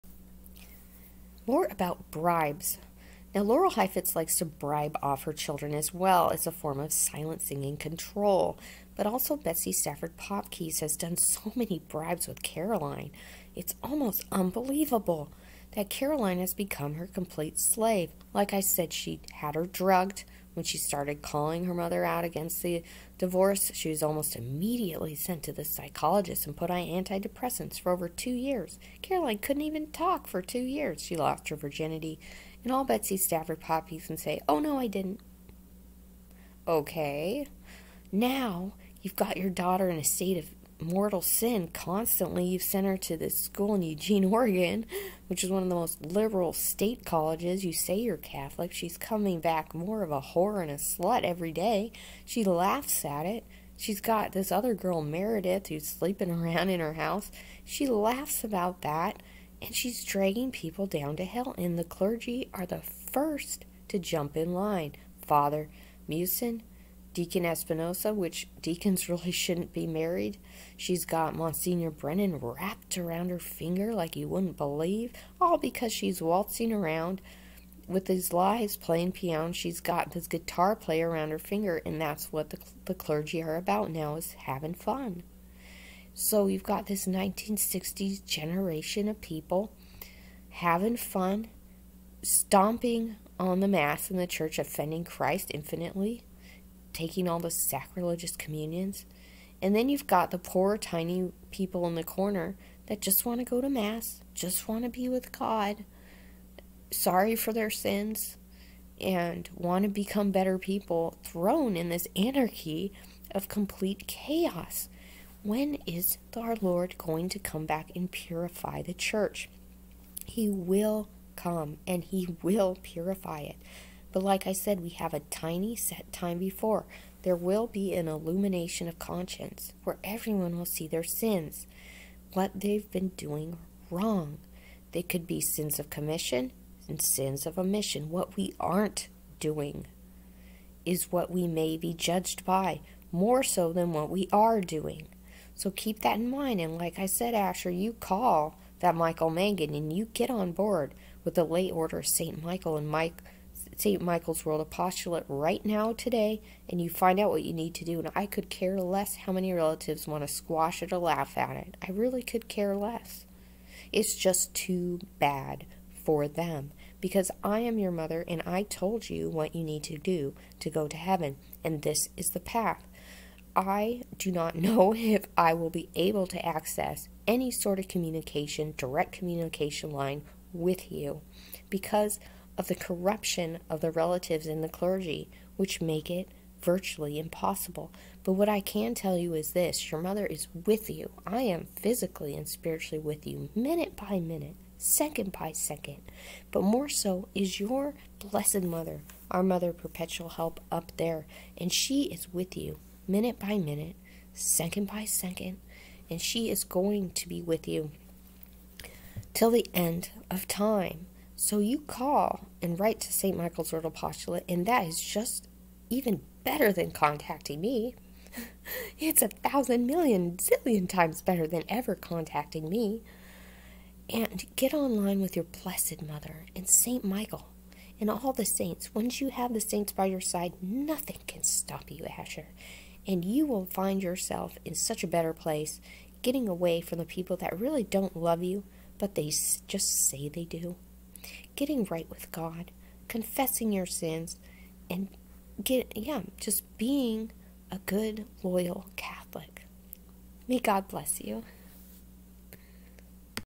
Betsy Stafford "Popkes" and the use of bribes and "buying" people off, including Steve Popkes.
Caroline Stafford has definitely been "bought off."